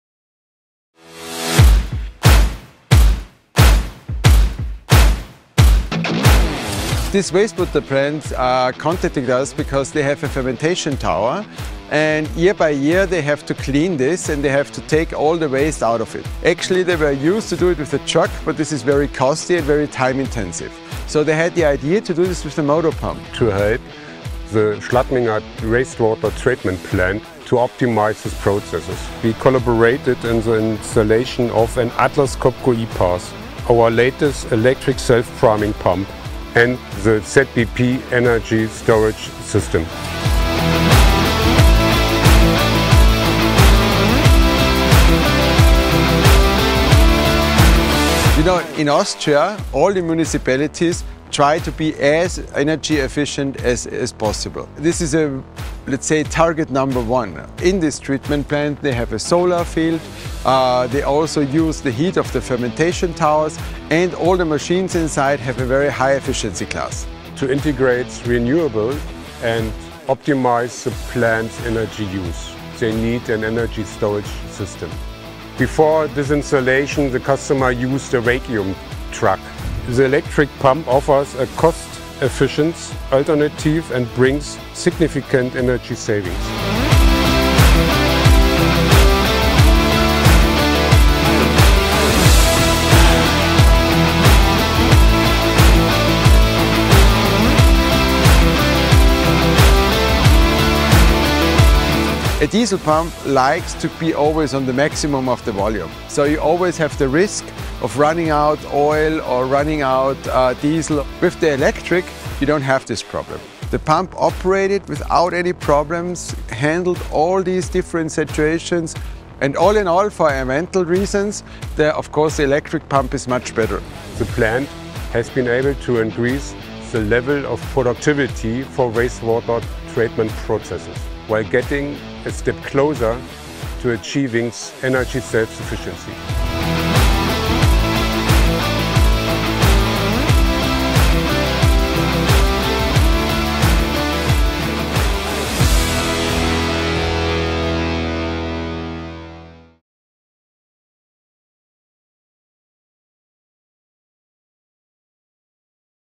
E PAS and ZBP energy storage system transform operations at Schladming wastewater treatment plant
For more information on Atlas Copco's range of self-priming electric pumps

To comply with environmental regulations and safeguard the natural beauty of the area, Schladming authorities were keen to minimise any environmental impacts from the local wastewater treatment plant. They also wanted to future-proof the treatment plant so it could be used by multiple local municipalities. Using an Atlas Copco energy storage system and electric pump, they developed a wastewater treatment plant that meets their sustainability goals.

The E PAS self-priming electric pump performs two functions: it makes removal of the waste from the digester tower much faster, easier, and cheaper, and also acts as a standby pump for other pumps at the treatment plant when they experience an outage for maintenance or repair.

In addition, to manage the power supply of certain operations, the plant has relied on a ZBP energy storage system from Atlas Copco, which can accumulate and deliver renewable energy from the plant's solar array.